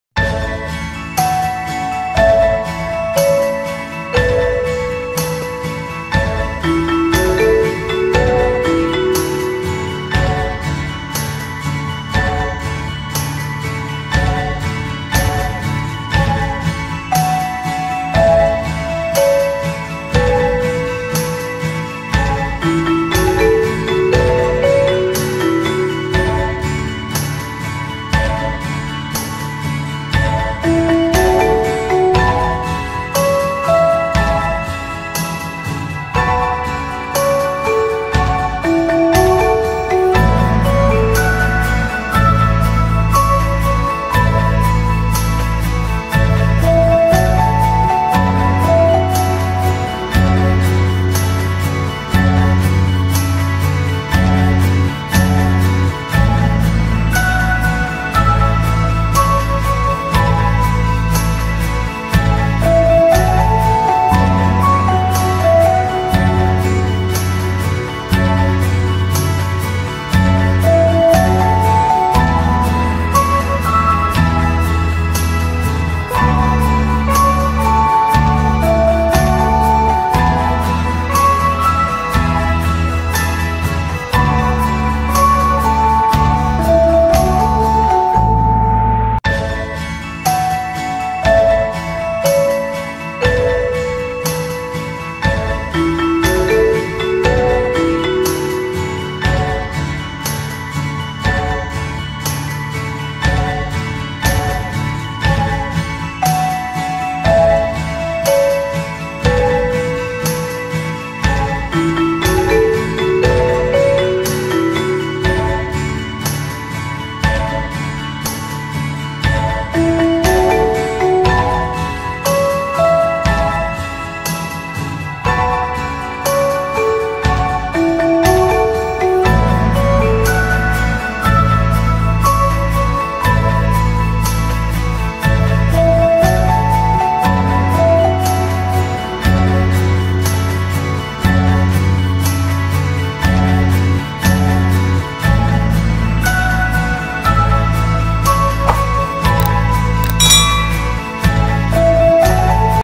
അടിച്ചുപൊട്ടിക്കുന്ന സർപ്രൈസ് കേക്ക് ഉണ്ടാക്കിയാലോ|Pinata Cake Without Mould |Surprise Cake
Ingredients :-

White chocolate
surprise gifts (choose any items. chocolate, cakes, etc. )